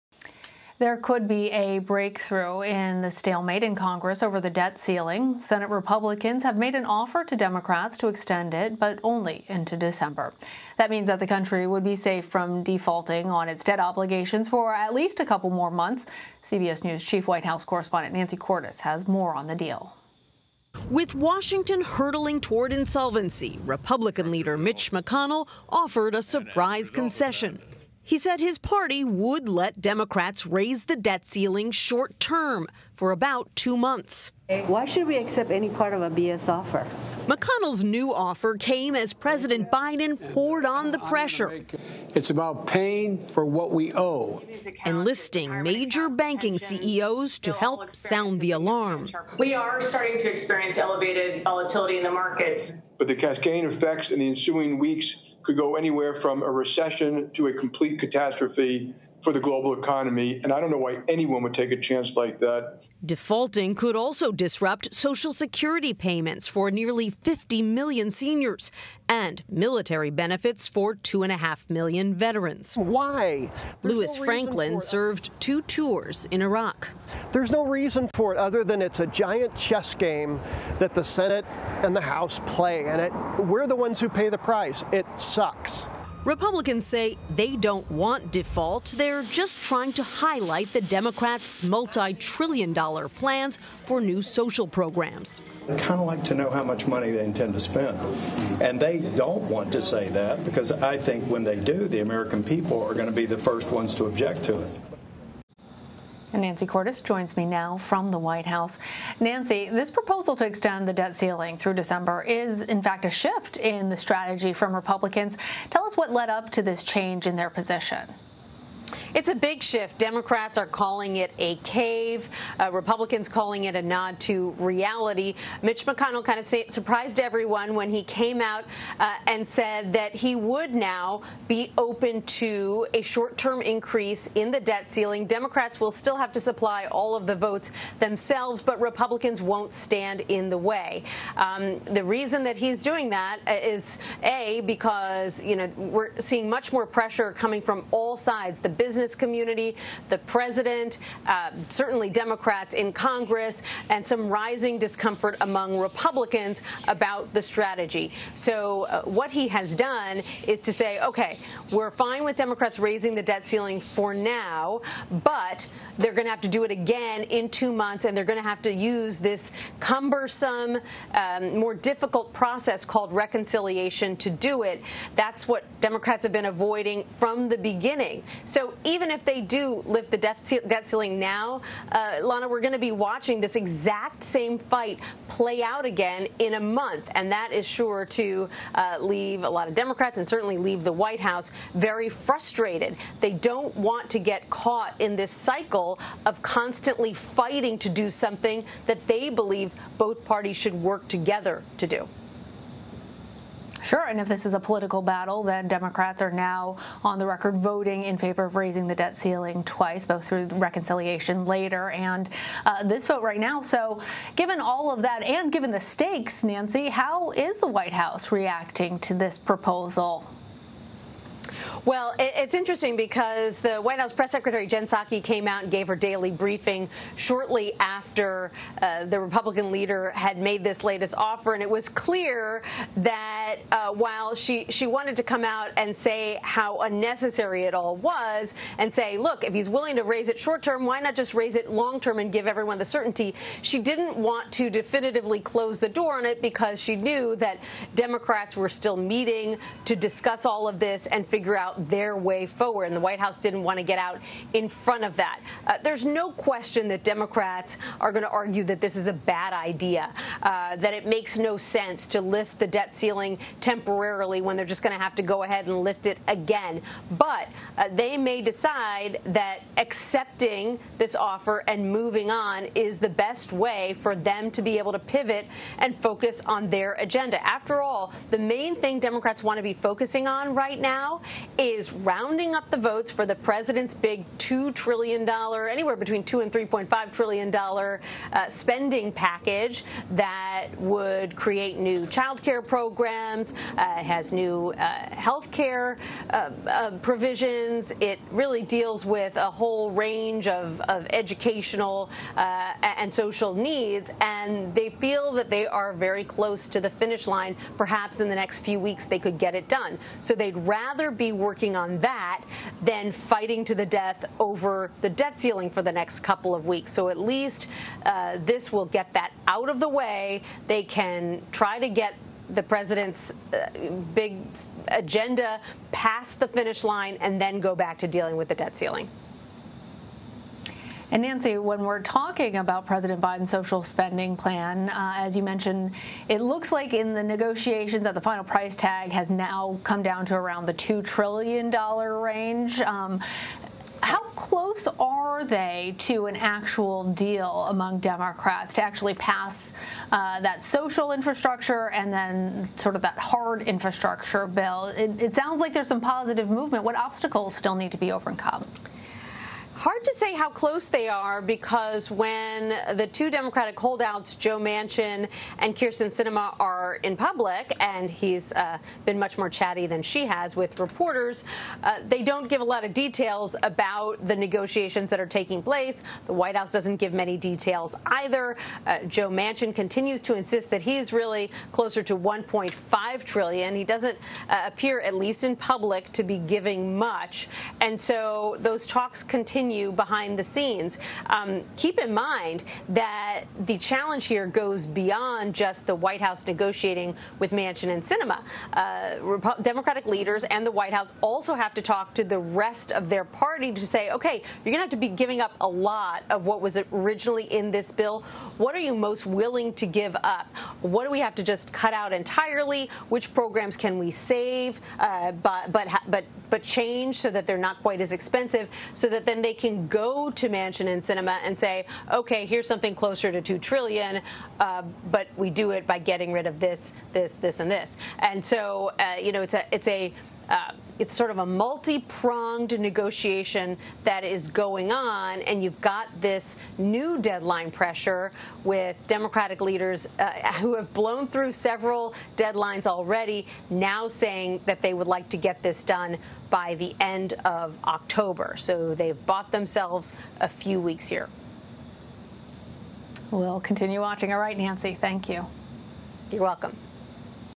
GOP offers Democrats a short-term debt ceiling extension
Senate Minority Leader Mitch McConnell offered Democrats a short-term solution that would extend the debt ceiling into December. CBS News chief White House correspondent Nancy Cordes breaks down the offer then discusses next steps with CBSN anchor Lana Zak.